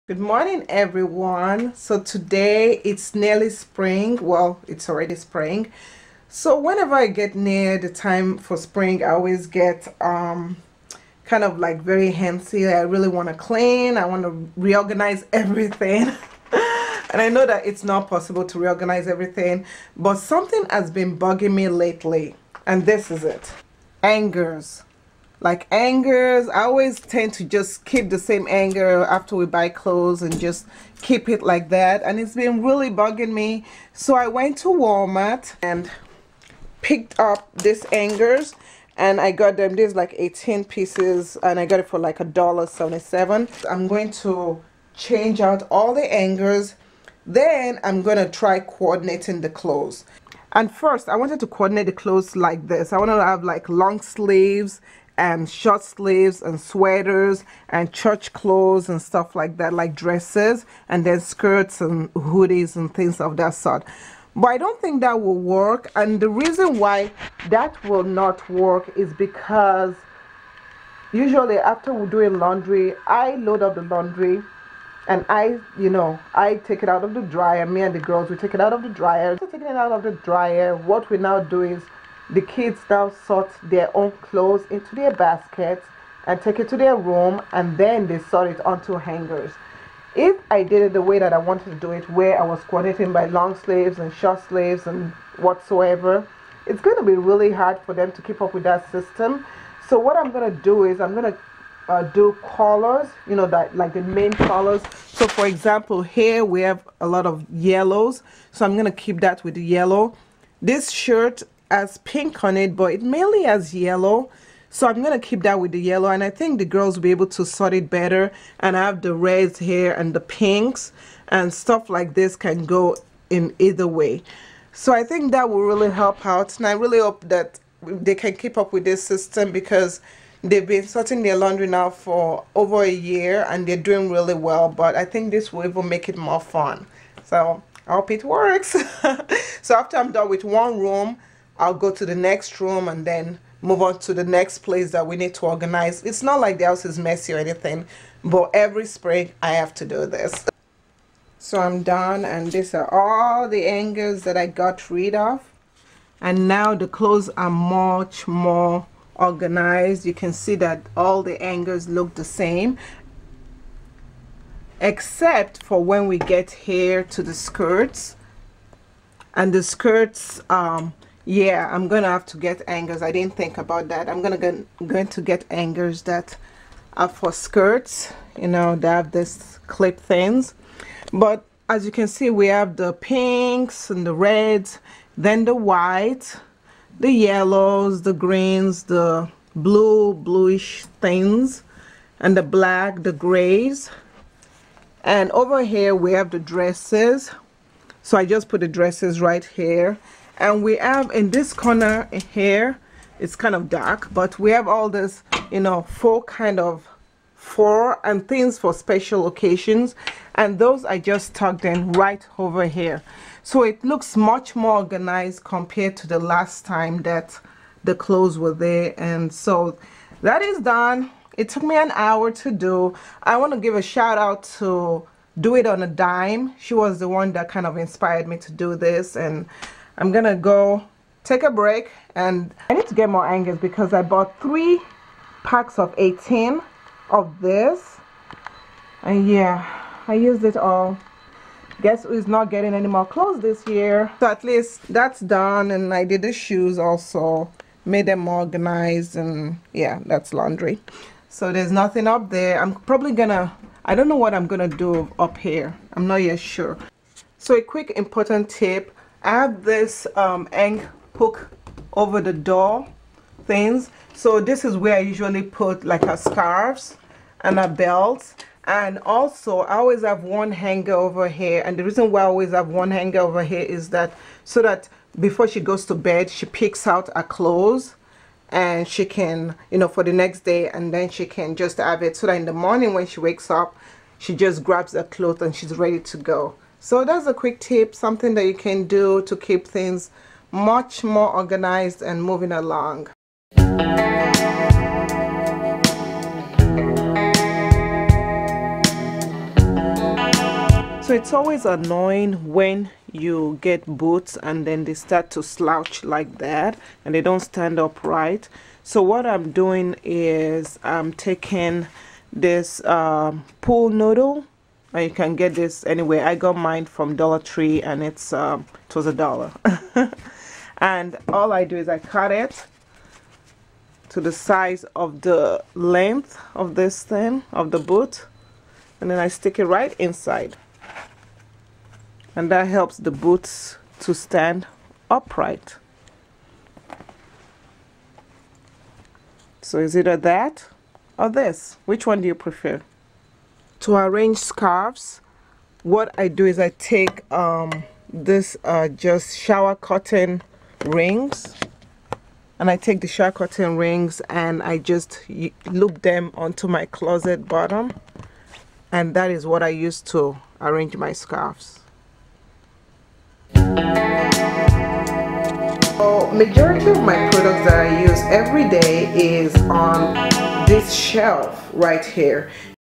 How to Organize Closet For Kids | March 26th 2014 DNVlogsLife
My kids sort their own laundry but the closet is always a mess. In this video, I show you a EASY way of organizing the closet and some COOL tricks

L i n k s Y o u W o u l d L O V E:

V i d e o E d i t i n g